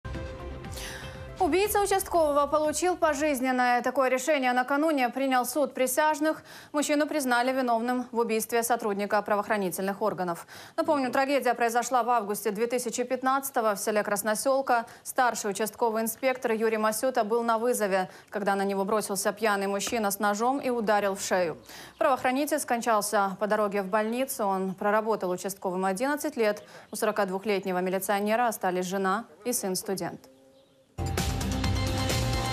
Убийца участкового в Одесской области получил пожизненное
Такое решение накануне принял суд присяжных – мужчину признали виновным в убийстве сотрудника правоохранительных органов при исполнении им служебных обязанностей.

Об этом сообщают в пресс-службе Прокуратуры в Одесской области.

Напомним, трагедия произошла в августе 2015 года в селе Красносёлка. Старший участковый инспектор Юрий Масюта был на вызове, когда на него бросился пьяный мужчина с ножом и нанес удар в шею. Правоохранитель скончался по дороге в больницу. Он проработал участковым 11 лет. У 42-летнего милиционера остались жена и сын-студент.